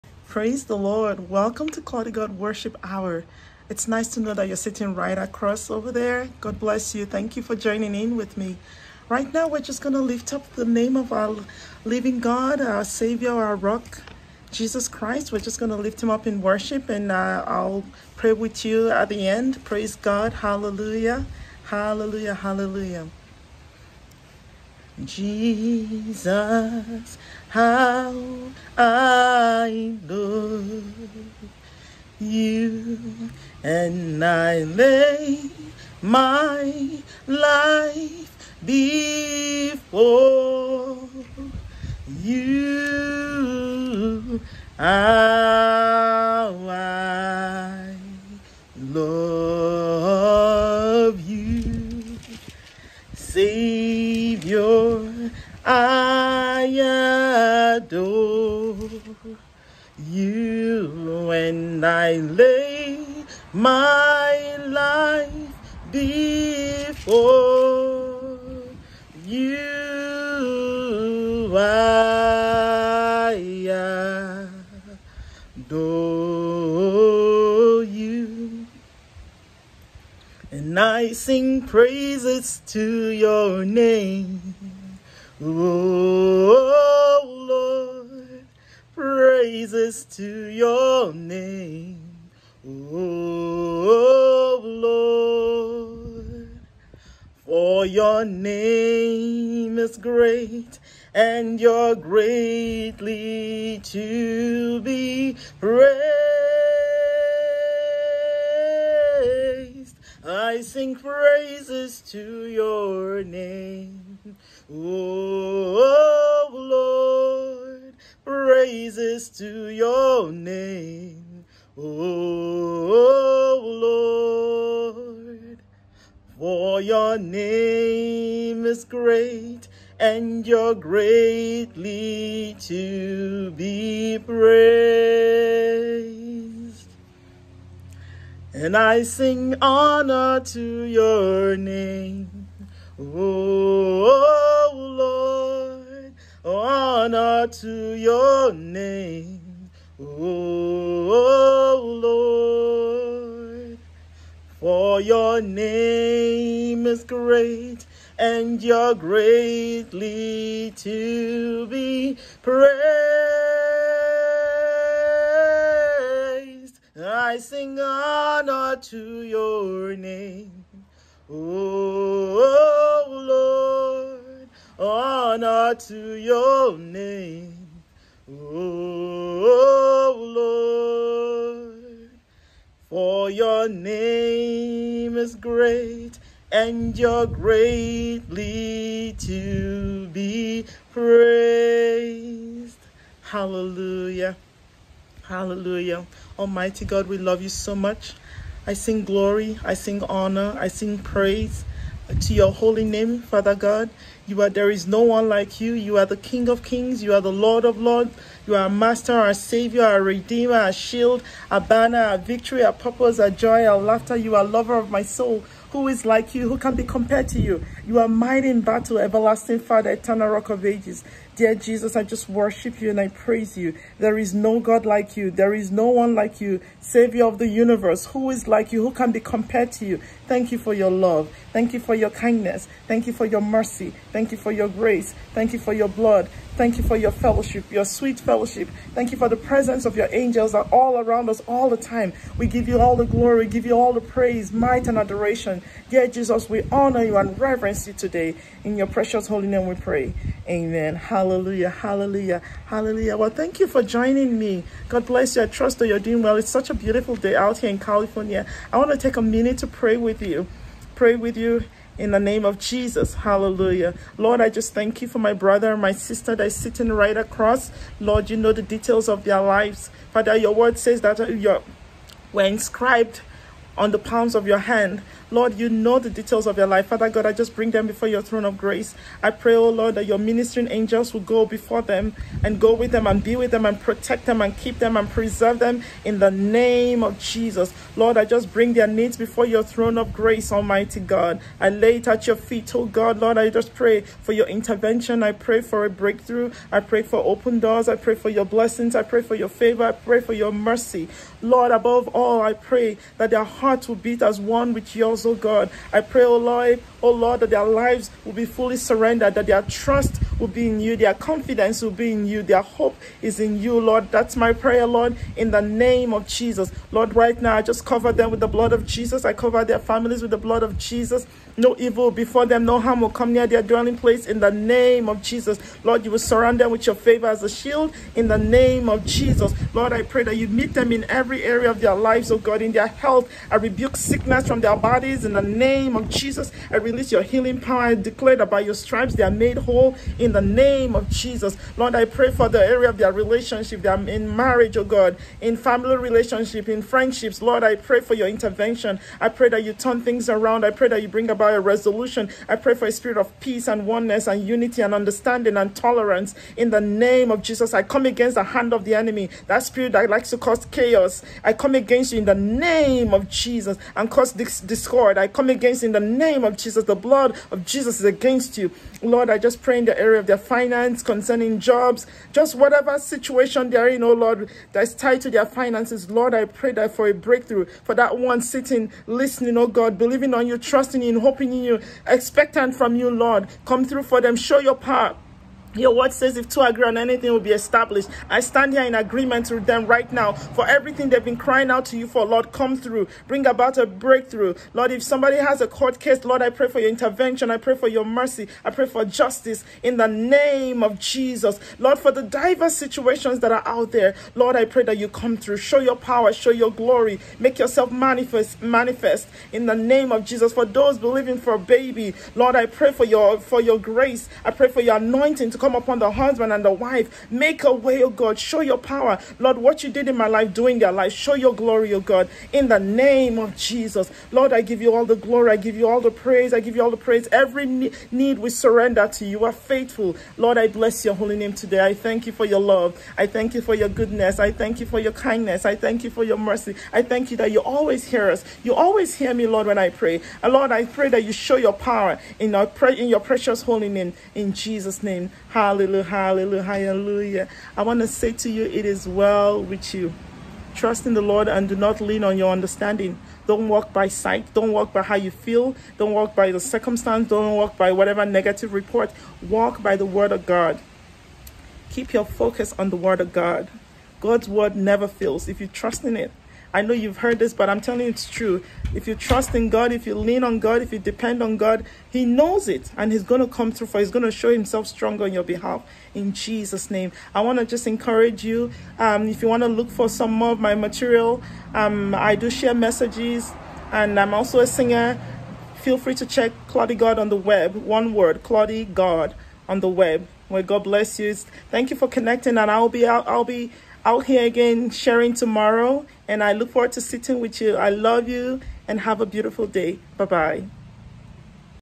Welcome to ClaudyGOD Worship Hour!
Join in and let’s worship God for just a few minutes. I’ll be praying for you at the end of the worship! Love you and God bless🙏